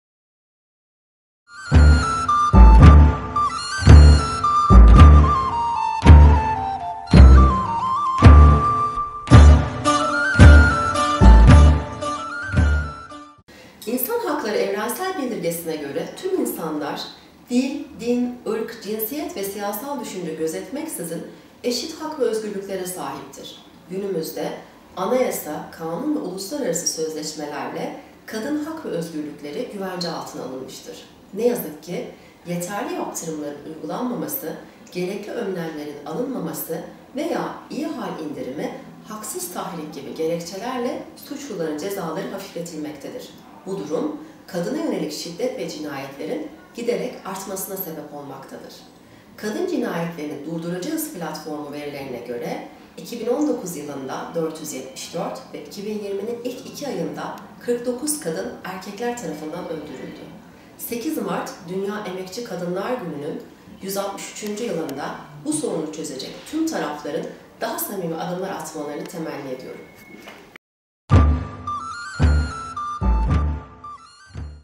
EMO ANKARA ŞUBESİ YÖNETİM KURULU ÜYESİ BİLGE ALĞIN’IN 8 MART DÜNYA EMEKÇİ KADINLAR GÜNÜ ÇAĞRISI
EMO Ankara Şubesi 25. Dönem Yönetim Kurulu Üyesi Bilge Alğın, 8 Mart Dünya Emekçi Kadınlar Günü dolayısıyla yaptığı çağrıda , İnsan Hakları Evrensel Bildirgesi, Anayasa, kanun ve uluslararası sözleşmeler ile kadın hak ve özgürlüklerinin güvence altına alındığının altını çizerek “Kadın Cinayetlerini Durduracağız platformu verilerine göre 2019 yılında 474 ve 2020’nin ilk iki ayında 49 kadın erkekler tarafından öldürüldü. `8 Mart Dünya Emekçi Kadınlar Günü`nün 163. Yılında bu sorunu çözecek tüm tarafların daha samimi adımlar atmalarını temenni ediyorum.” dedi. Bilge Alğın’ın çağrısını haberin devamından izleyebilirsiniz.